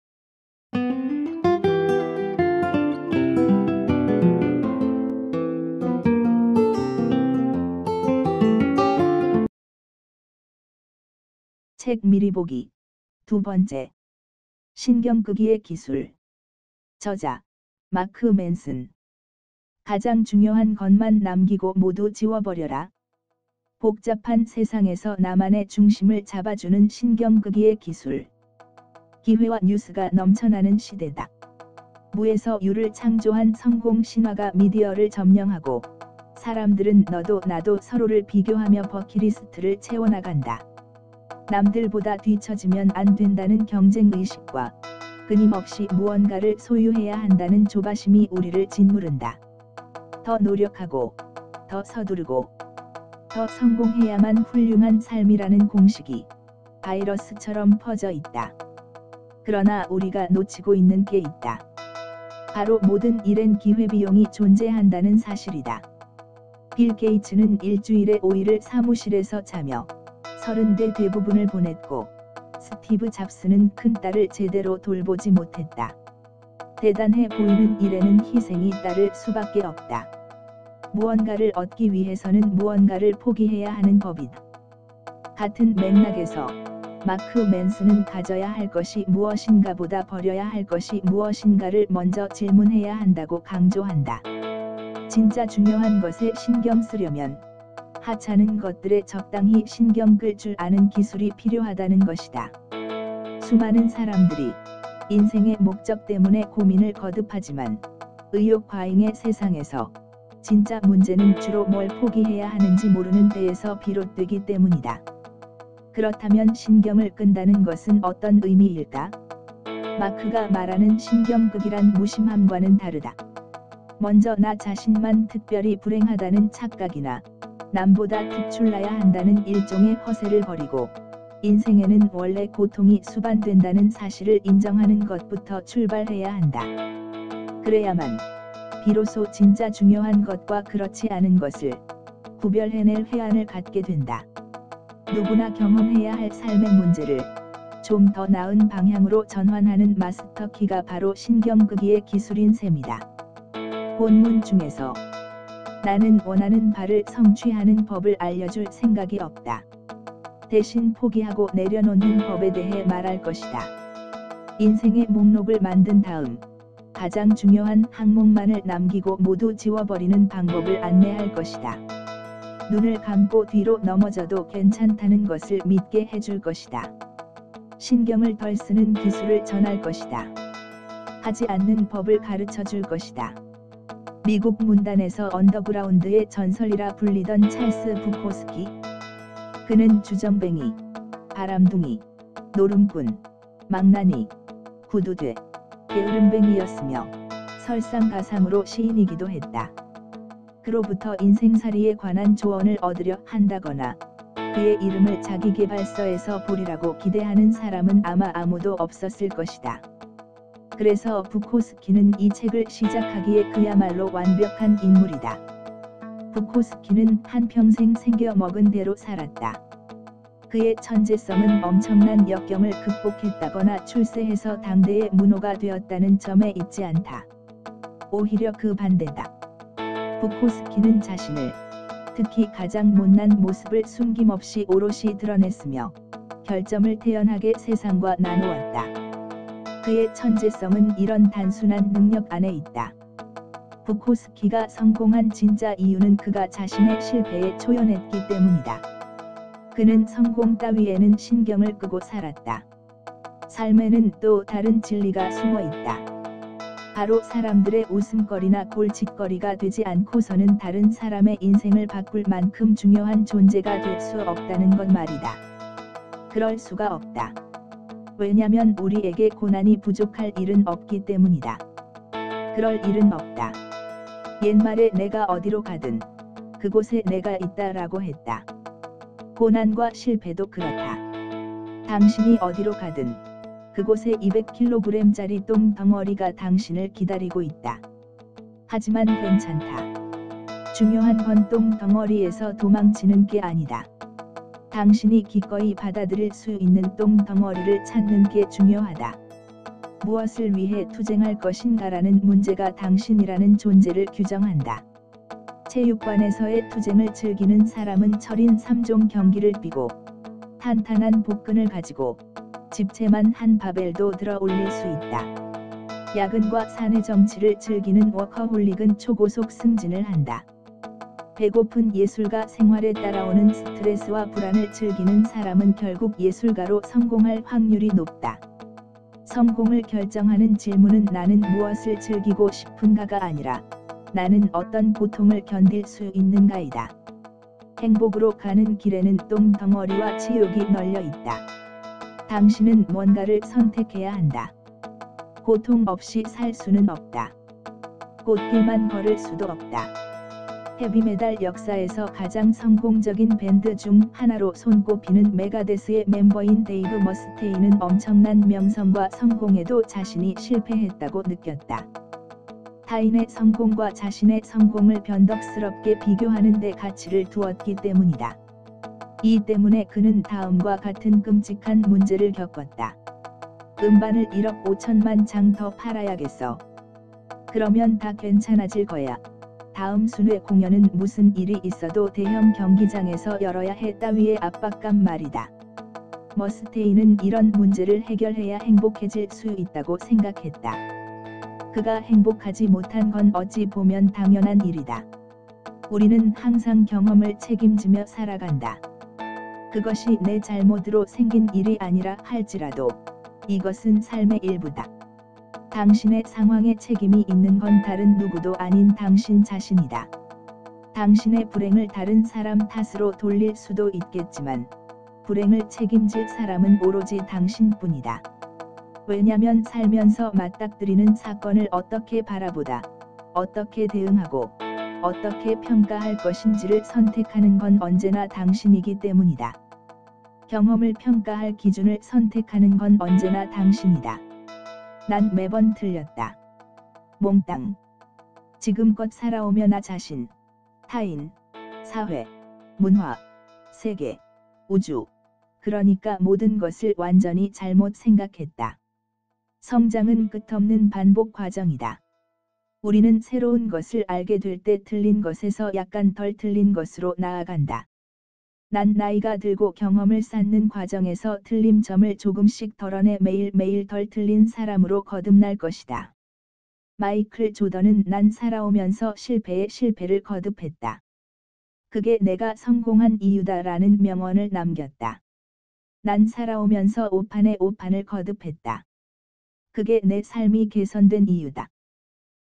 신경끄기의 기술 - 성공하려면? : 애쓰지마! 노력하지마! 신경쓰지마!
책 미리보기 - 두번째  [신경끄기의 기술] - 마크 맨슨  저
책 미리보기를 더 보시려면 구독은 필수!

성공하려면 애쓰지 마라는 당돌한 책이 나와서 화제다.
세상에 신경 쓸 일이 너무 많아서 이 것 저 것 신경쓰다가 인생을 낭비하는 일이 많다는 소리다.